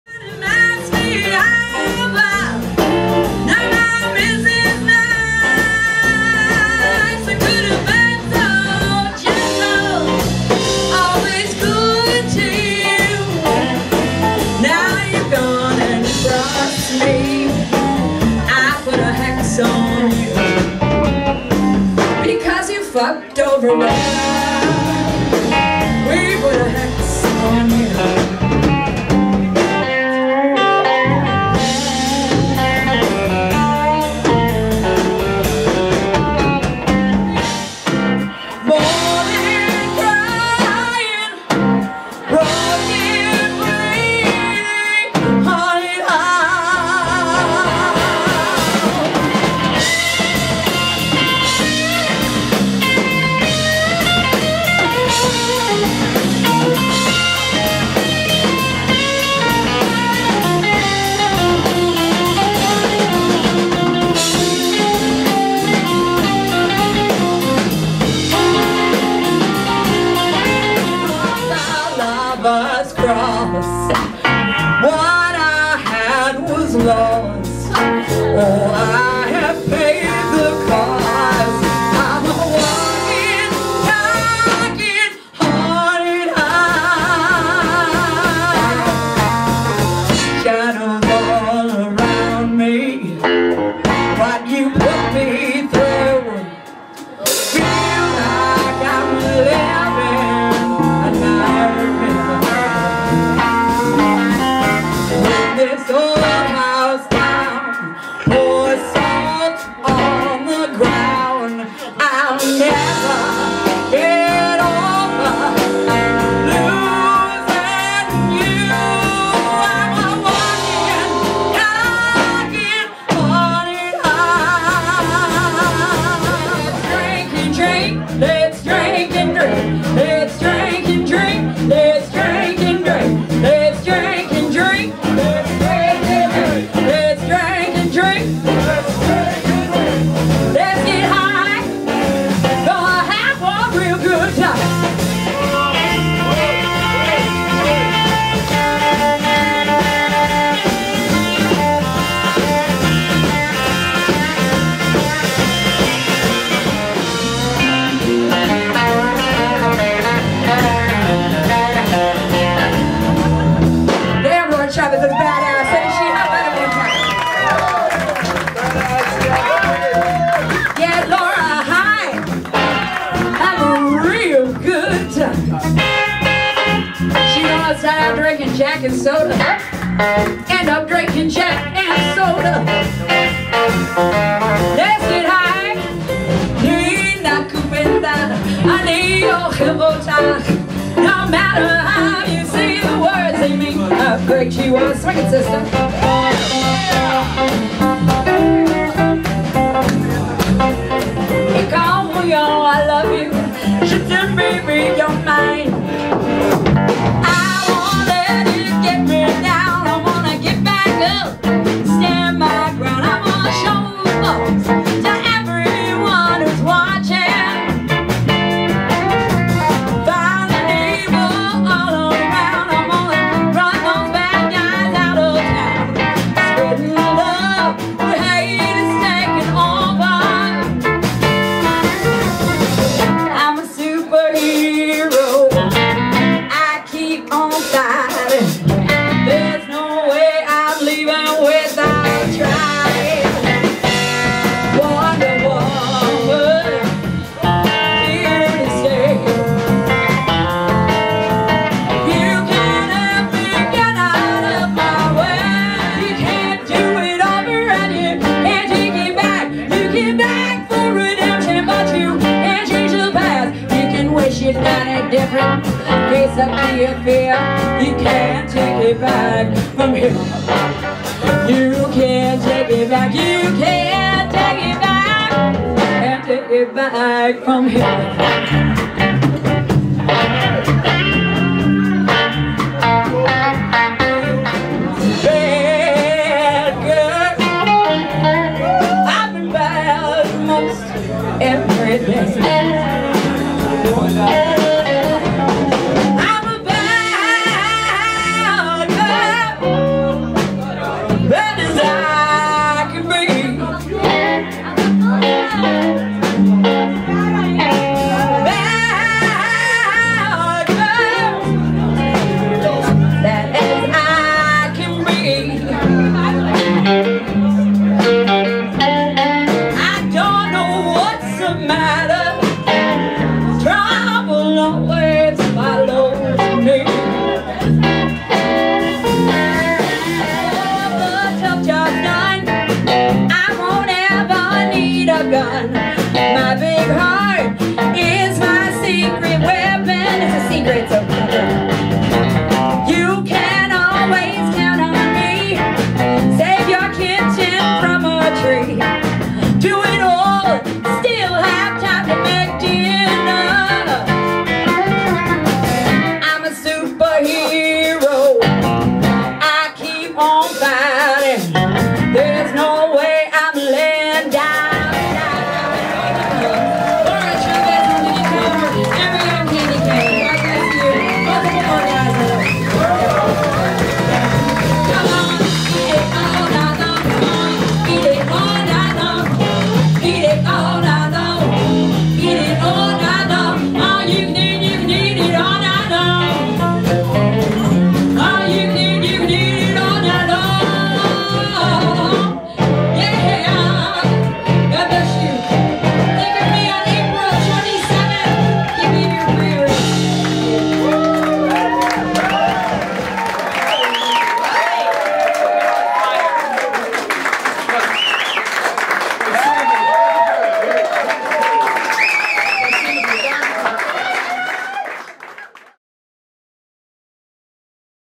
Candye Kane. Meloe Melo
on Europe tour 2012, Candye Kane , L.C. and band.

Amsterdam Blues cafe "Meloe Melo"